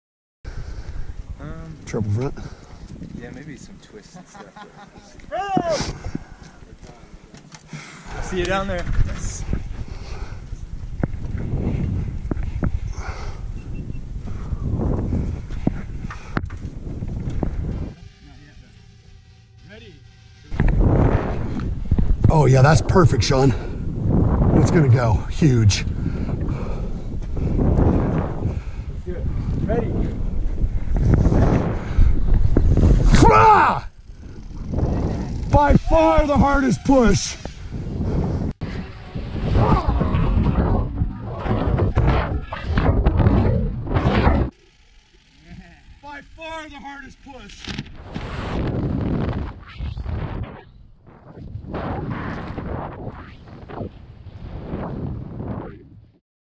Russian Swing BASE Jump
Sean Chuma jumping from the new and improved Russian swing, which was engineered by Travis Tripp.  The swing was at a location in southern Utah.  Sean and travis had set up the swing for the first time in 2015, marking the start in the new sport of Russian Swing BASE jumping. Here they return during thanksgiving 2018 to sample the power of this bigger and better swing.